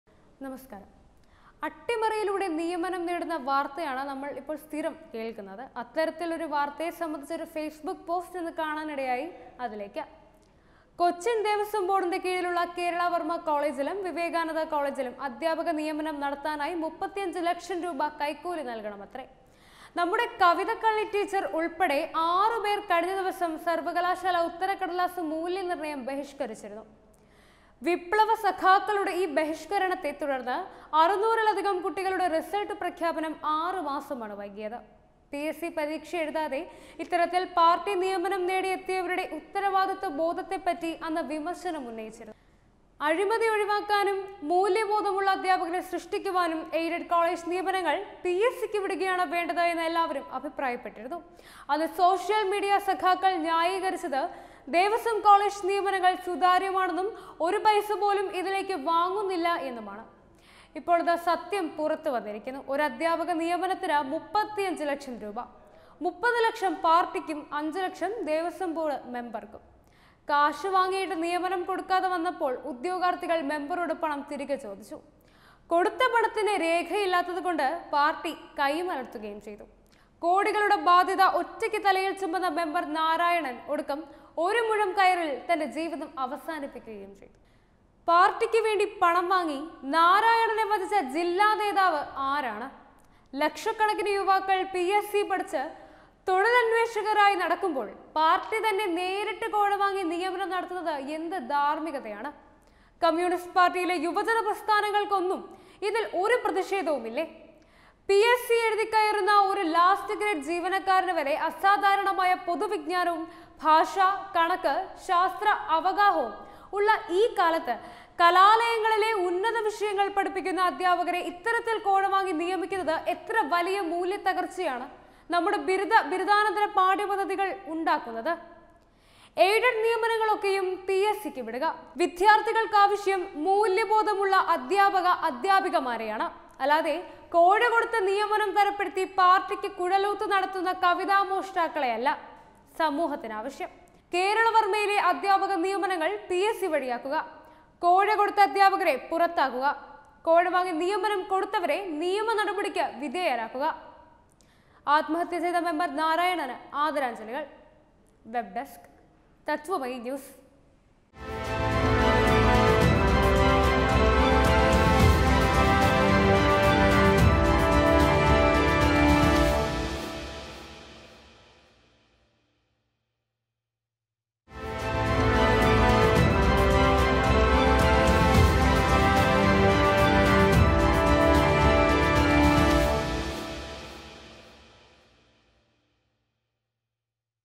പിഎസ് സി പരീക്ഷ എഴുതാതെ അട്ടിമറിയിലൂടെ നിയമനം, ഒരു അദ്ധ്യാപക നിയമനത്തിന് 35 ലക്ഷം, ഇതാ സത്യം പുറത്ത്
അട്ടിമറിയിലൂടെ നിയമനംകൈക്കൂലി 35 ലക്ഷം ഇത് തീർന്നില്ലേയെന്ന് സോഷ്യൽ മീഡിയ